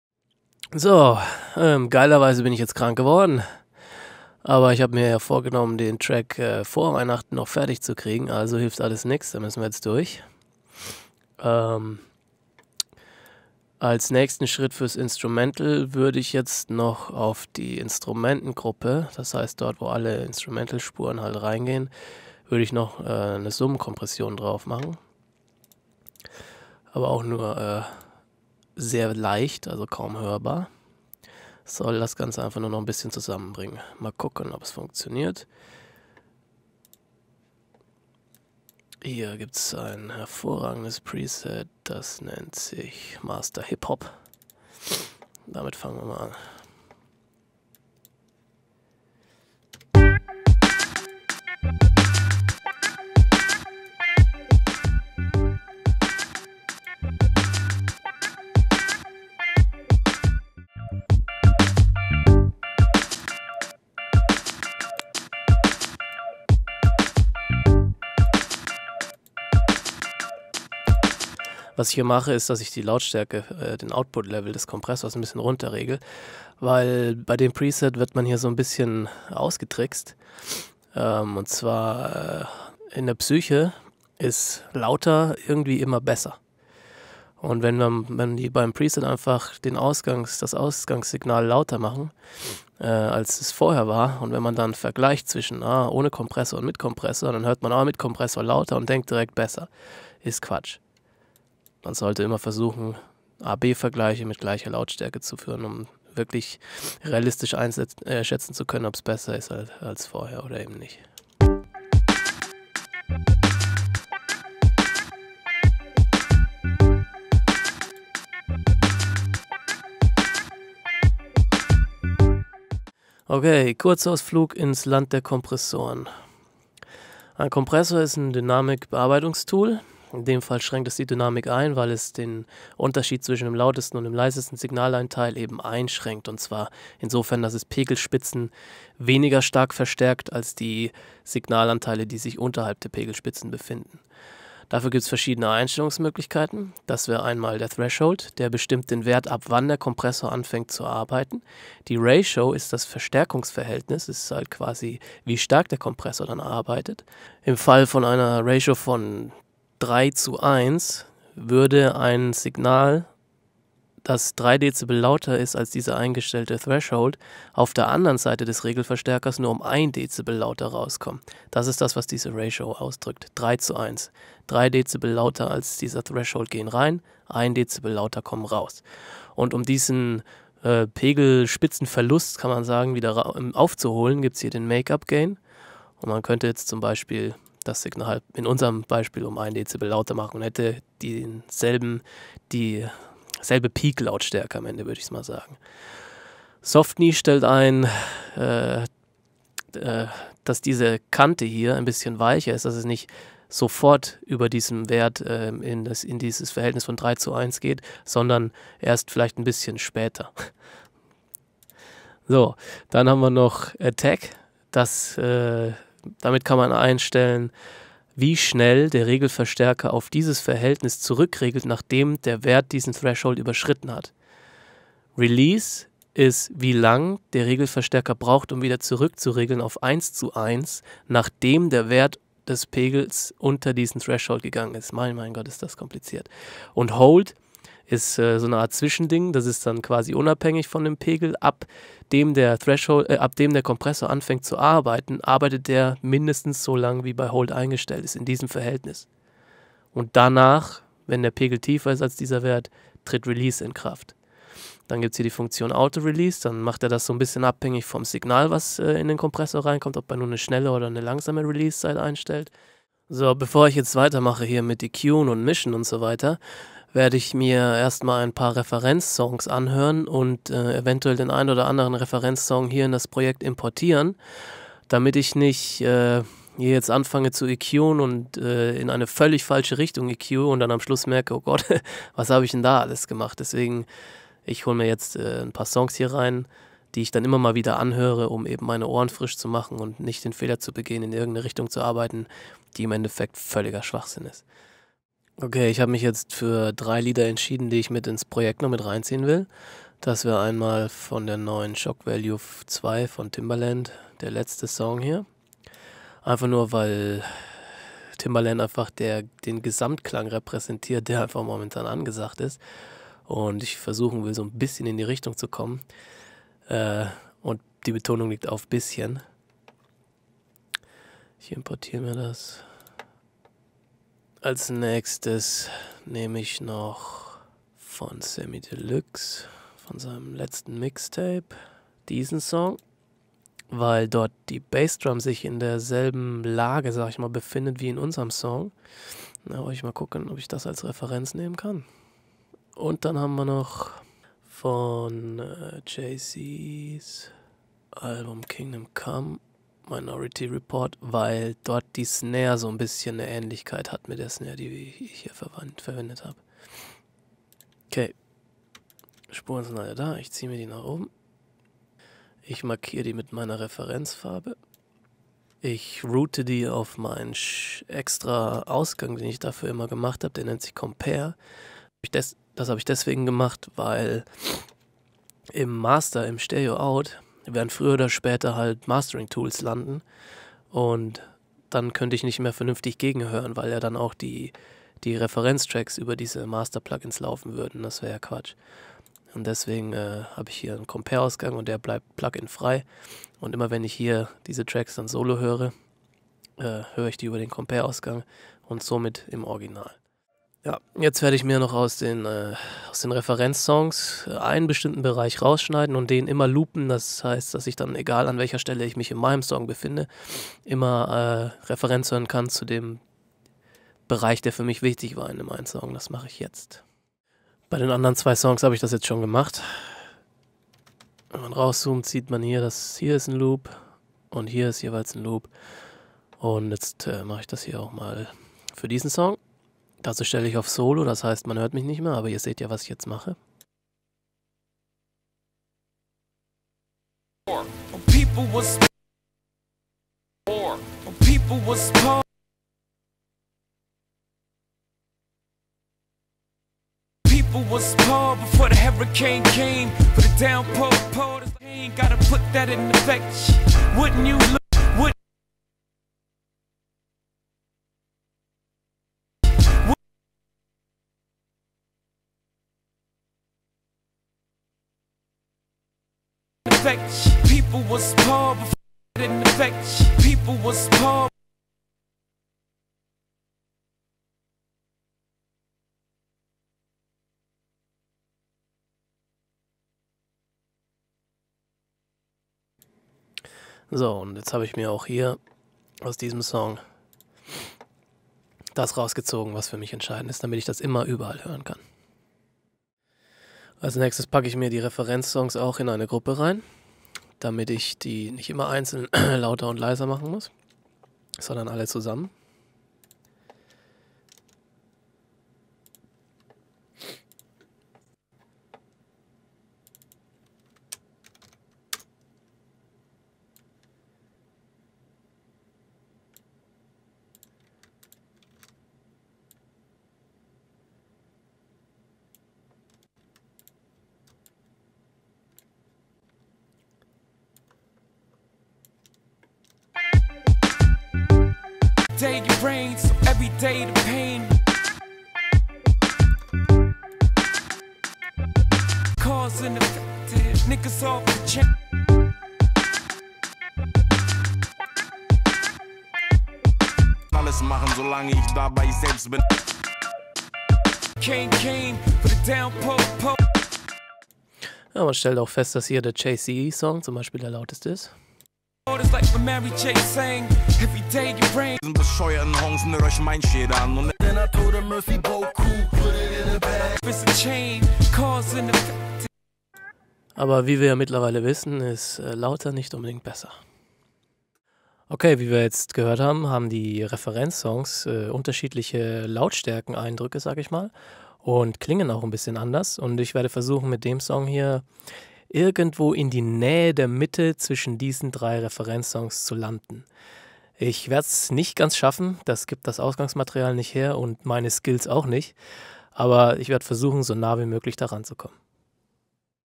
Arbeiten mit Cubase 5 - Hip Hop Mixdown 13
Kleine Doku über meine Arbeitsweise mit Cubase am Beispiel eines Hip Hop Session Tracks.
- Summenkompression des Instrumentals
- Erkärung der Parameter eines typischen Kompressors
- Aussuchen & Importieren von Referenz-Songs
- Erstellen von Referenz Loops und Referenz-Gruppe